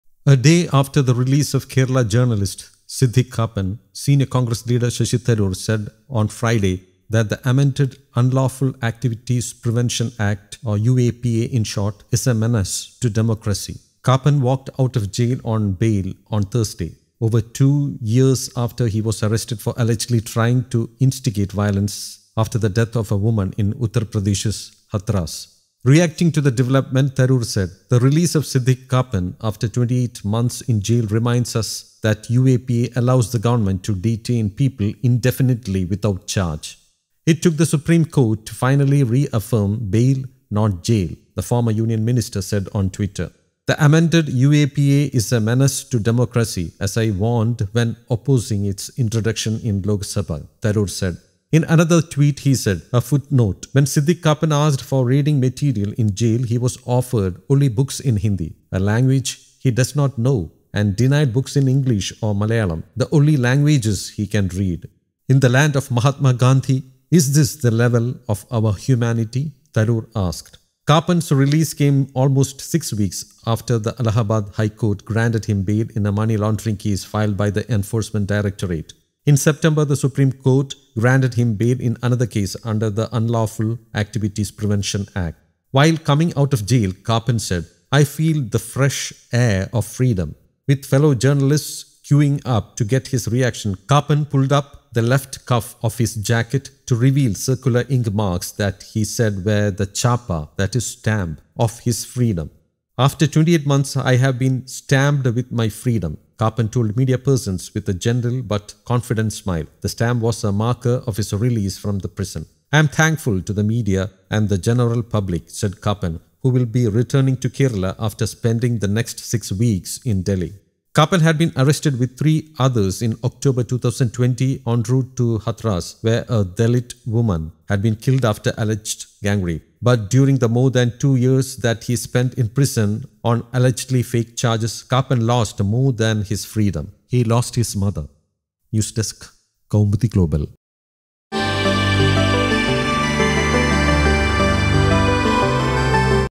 Kappan reveals 'chappa' on hand; Tharoor on Kappan’s release
New Delhi: A day after the release of Kerala journalist Siddique Kappan, senior Congress leader Shashi Tharoor said on Friday that the amended Unlawful Activities (Prevention) Act (UAPA) is a "menace to democracy". Kappan walked out of jail on bail on Thursday, over two years after he was arrested for allegedly trying to instigate violence after the death of a woman in Uttar Pradesh's Hathras.

Reacting to the development, Tharoor said, "The release of Siddique Kappan after 28 months in jail reminds us that UAPA allows the government to detain people indefinitely without charge."